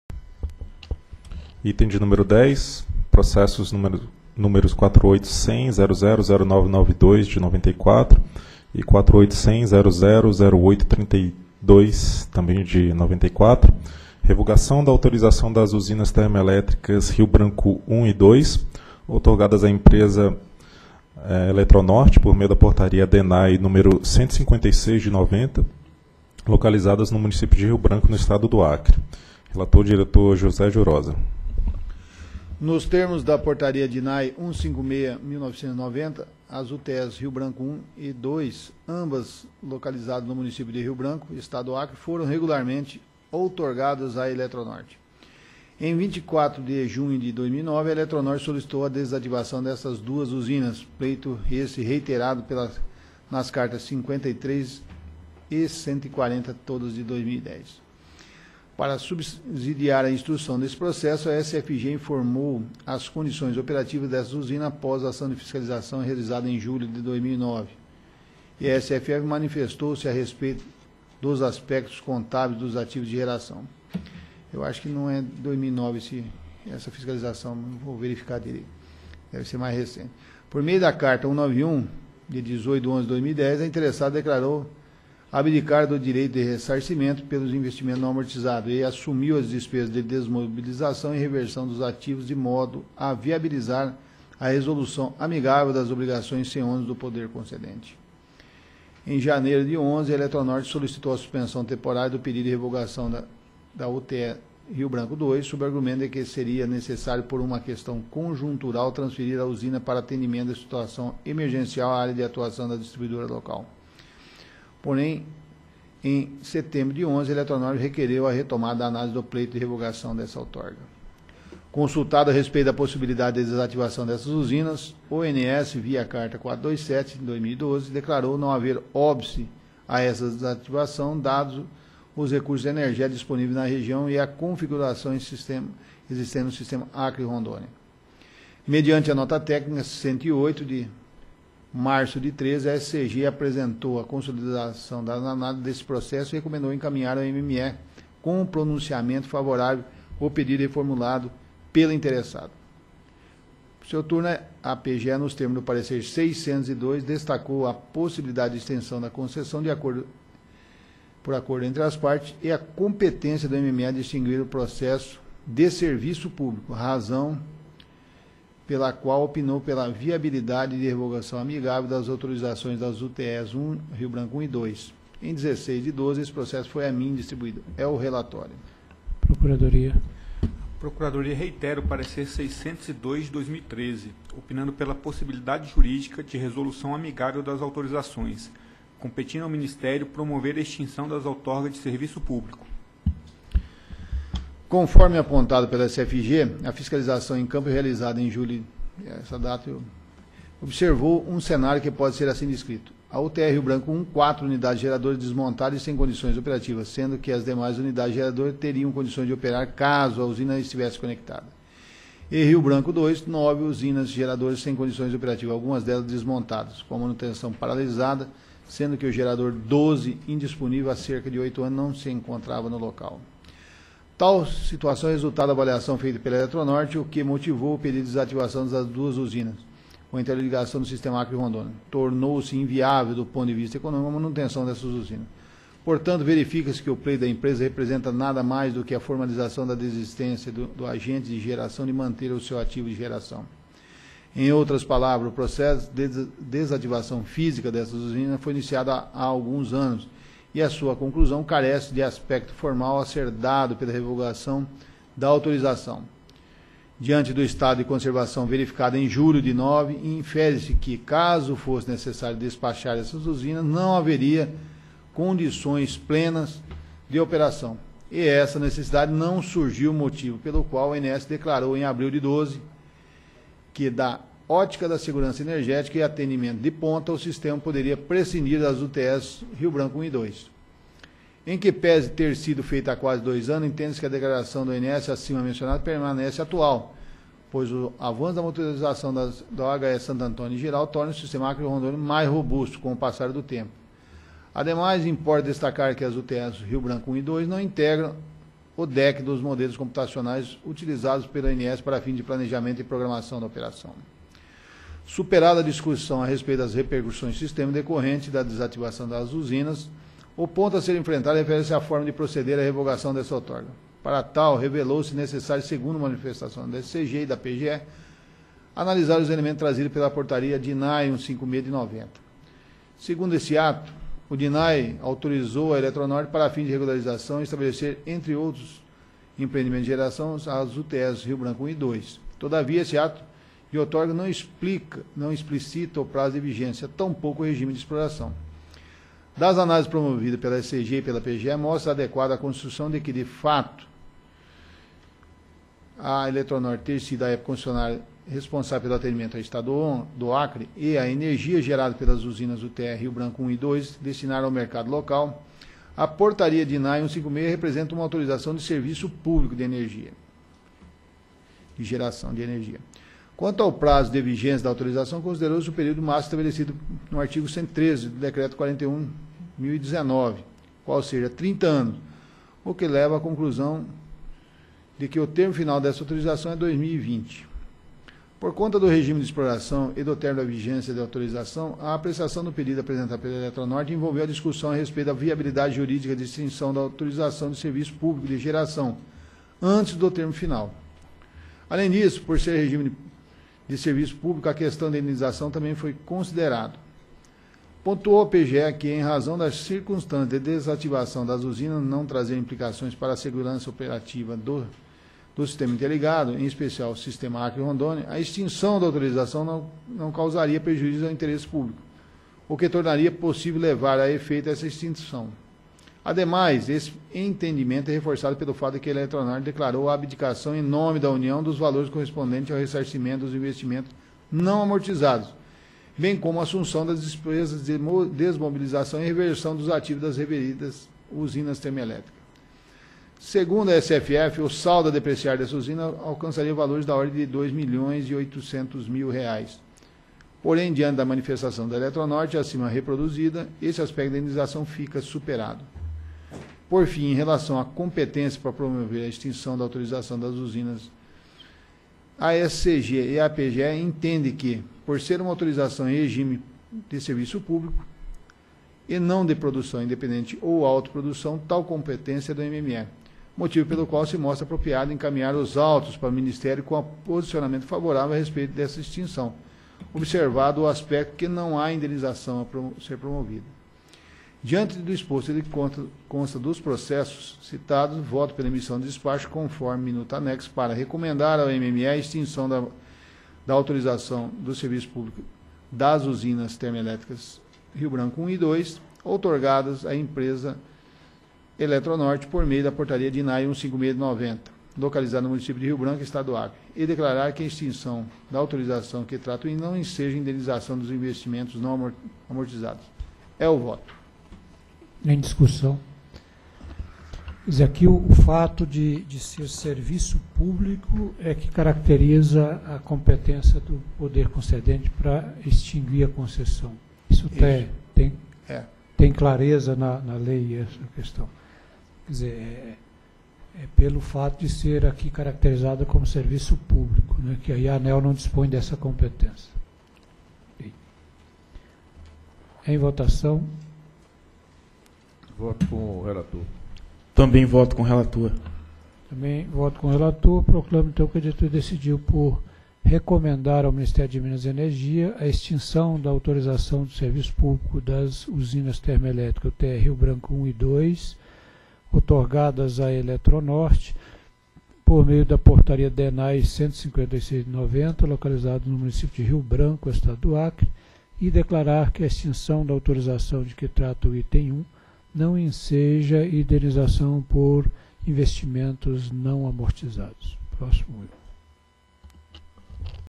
Item 10 - 1ª Reunião Pública Ordinária ANEEL 21/01/2014
Item 10. Processos: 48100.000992/1994-54 e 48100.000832/1994-88. Assunto: Revogação da autorização das Usinas Termelétricas -- UTEs Rio Branco I e Rio Branco II, outorgadas à empresa Centrais Elétricas do Norte do Brasil S.A. -- Eletronorte por meio da Portaria DNAEE nº 156/1990, localizadas no município de Rio Branco, estado do Acre. Relator: Diretor José Jurhosa Junior.